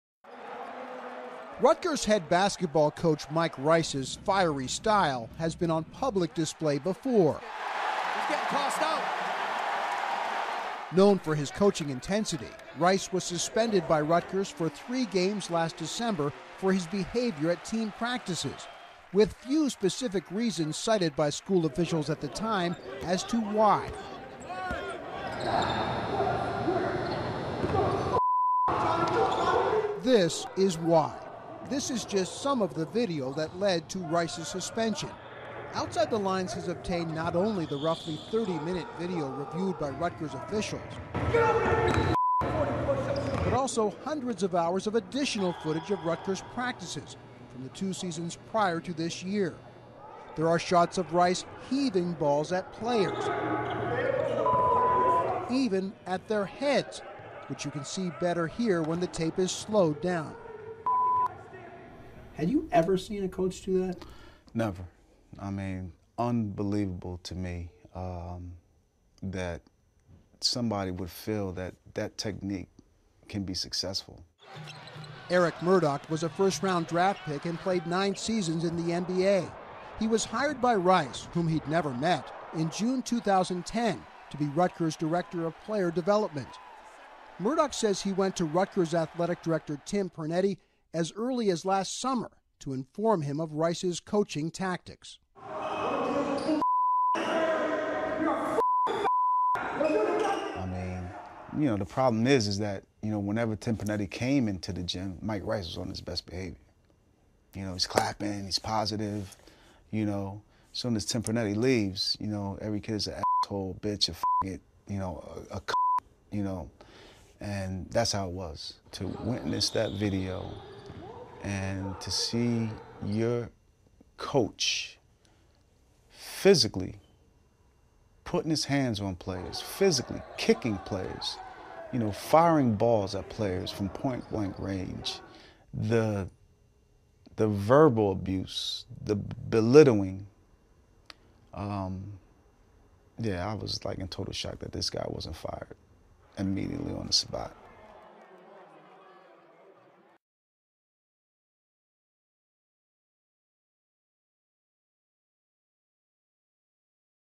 HD Rutgers Basketball Coach Mike Rice Abusing His Players 2013
Rutgers Basketball Coach "Mike Rice" Caught On Tape Abusing His Players